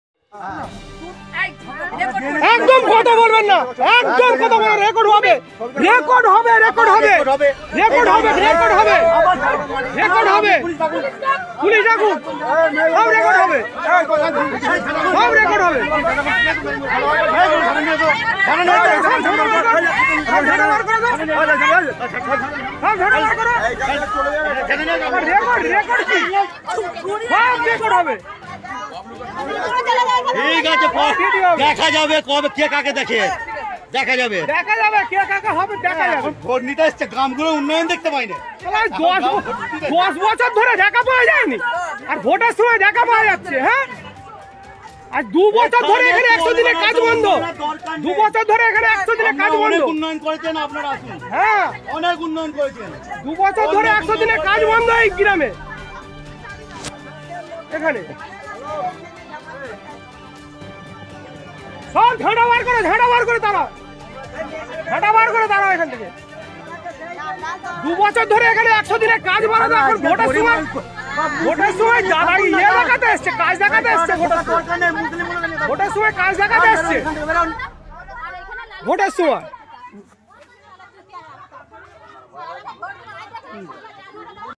टीएमसी उम्मीदवार असीमा पात्र को देखकर भड़के ग्रामीण, वीडियो वायरल Jai Hind Samachar
Hooghly hot